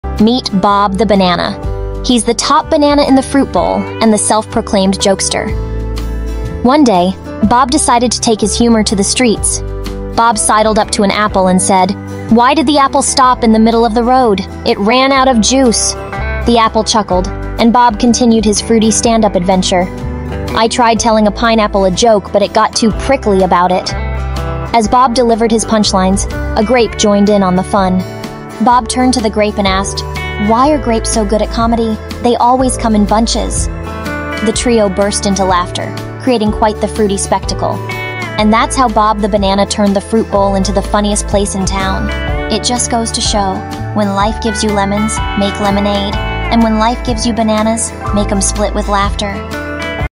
Juicy Jokes: Bob the Banana's Comedy Bonanza
🍌🎤 Get ready for a peel-good time with Bob the Banana in this hilarious YouTube Shorts series! 🤣🍇 Join Bob as he turns the ordinary fruit bowl into the funniest place in town with his fruity stand-up adventure. 🍎🤹‍♂️

In each episode, Bob sidles up to different fruits, delivering puns and jokes that will make you split with laughter. 🍑🤣 From plums to peaches, watermelons to coconuts, no fruit is spared from Bob's comedic charm. 🥭😂

Experience the contagious laughter as the fruit bowl comes alive with rolling, bouncing, and jiggling fruits, creating a fruity carnival of humor. 🍊🎉